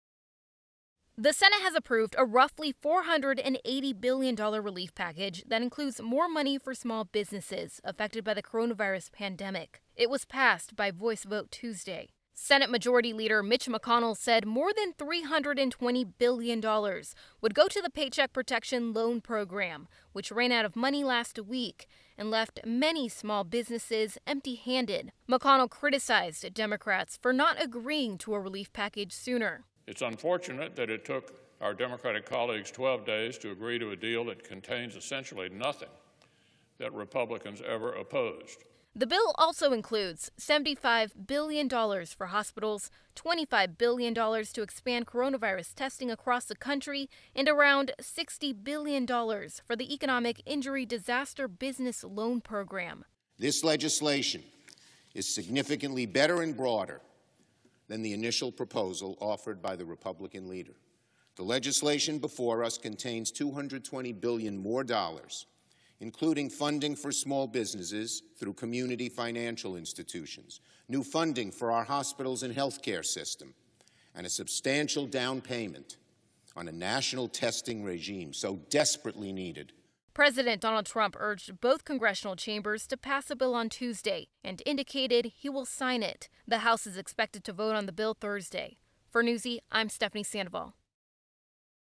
Senate approves coronavirus relief bill
More than $320 billion would go to the Small Business Administration's Paycheck Protection Program, which ran out of money last week.